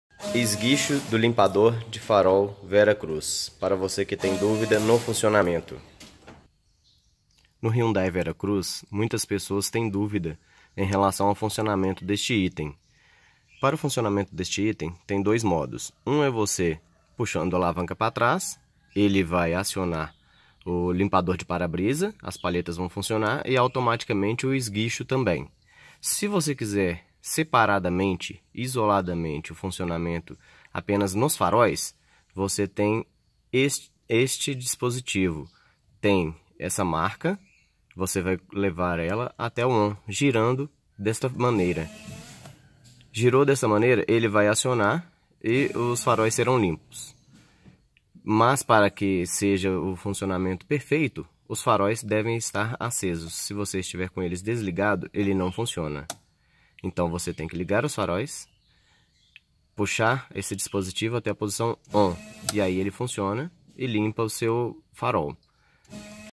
Lavador de Farois Hyundai Veracruz
Videp Explicativo de como usar o Lavador de Farois da Veracruz by Leandro Ramos (MG)